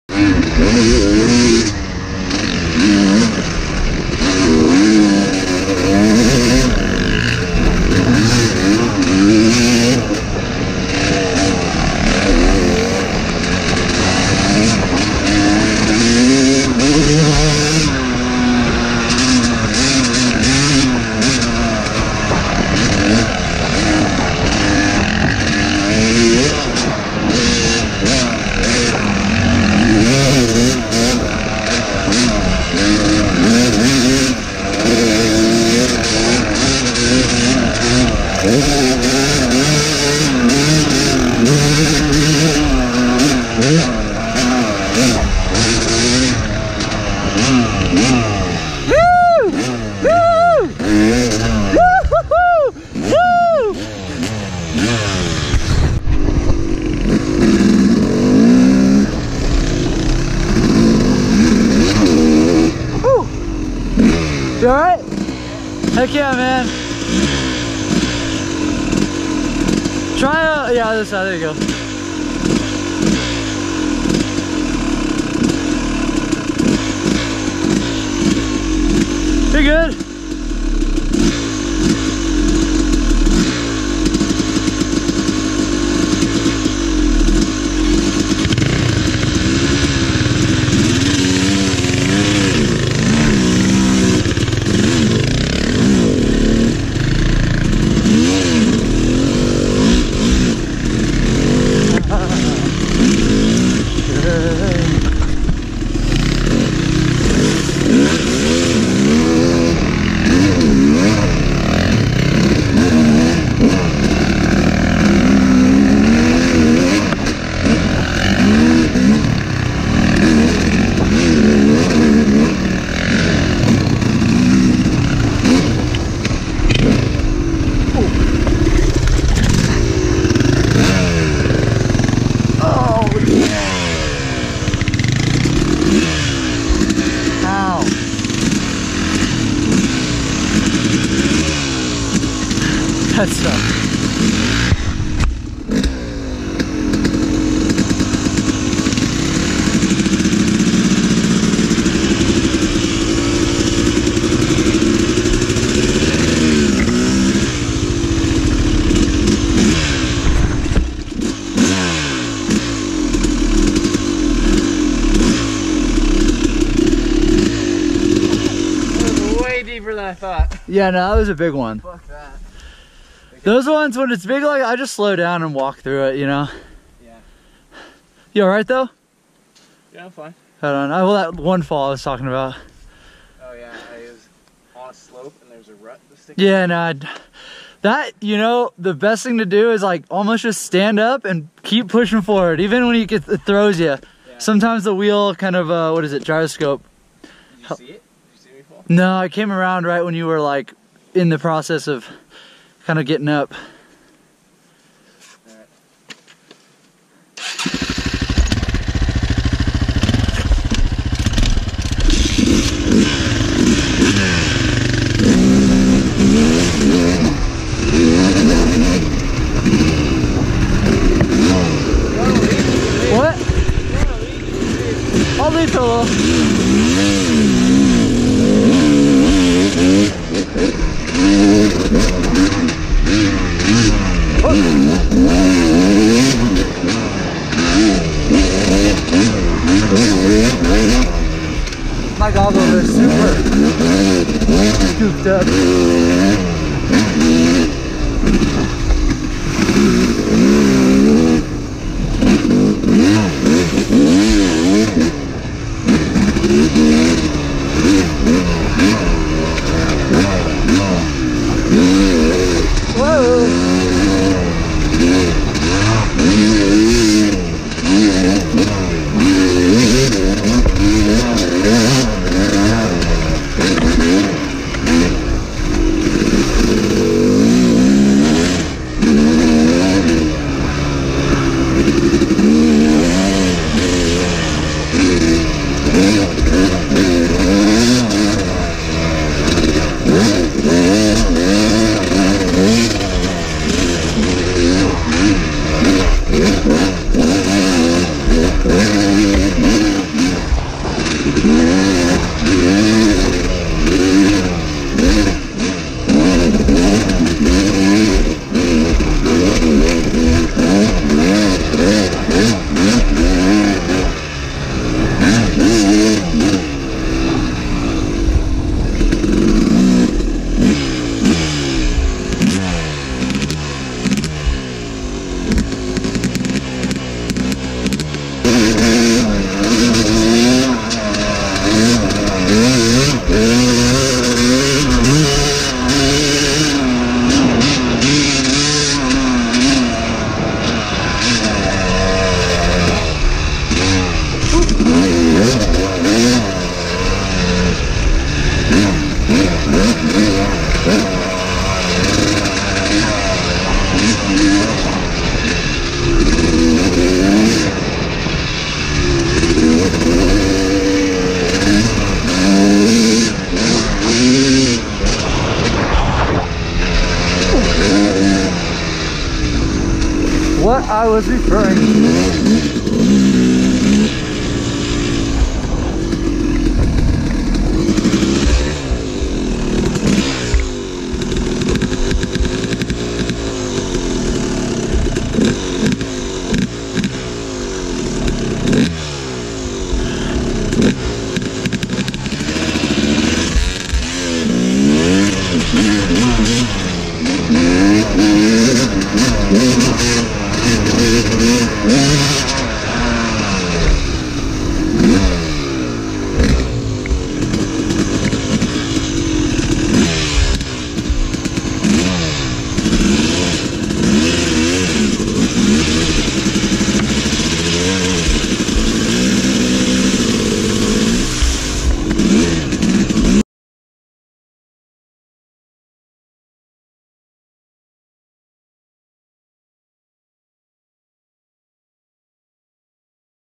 Super High Speed Pass (Yz 250 Passing 450)
YZ 250 vs KTM 450 in the woods battling. This was a really fun day!

Business IG: senorsnipes
personal IG: I_love_yoga_pants

HOBBIES?